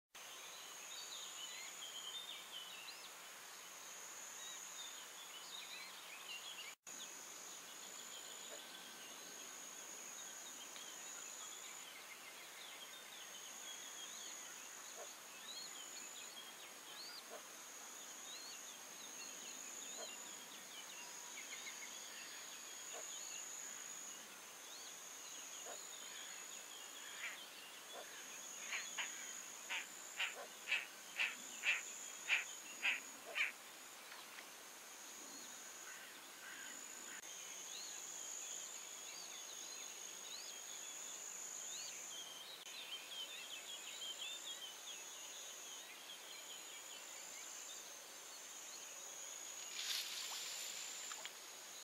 Tarabusino (Ixobrychus minutus) nel canneto
IT: Suoni e colori del canneto con Tarabusino (Ixobrychus minutus). Versione a bassa risoluzione.
EN: Sounds and colors of the reed bed with Little Bittern (Ixobrychus minutus). Low resolution version.